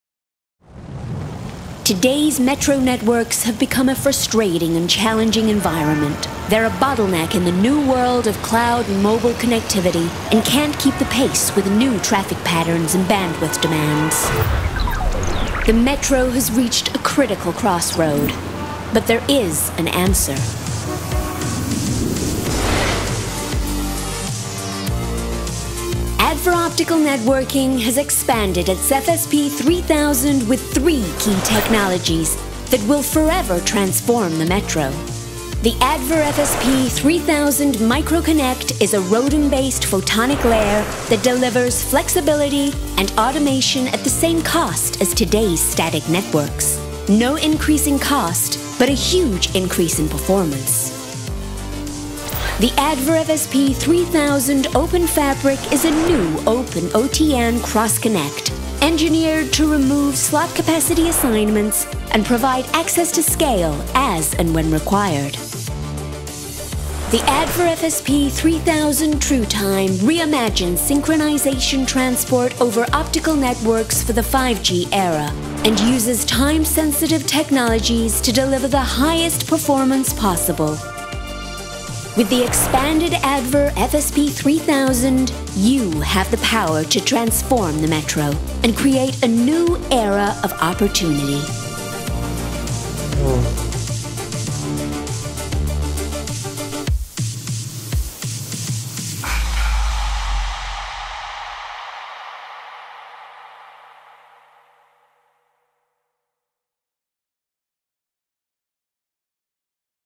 Transforming Metro Networks
Our expanded FSP 3000 features three new technologies that will forever transform the metro. Network operators can now meet the rapidly changing needs of the metro environment with unprecedented levels of flexibility, scale and synchronization.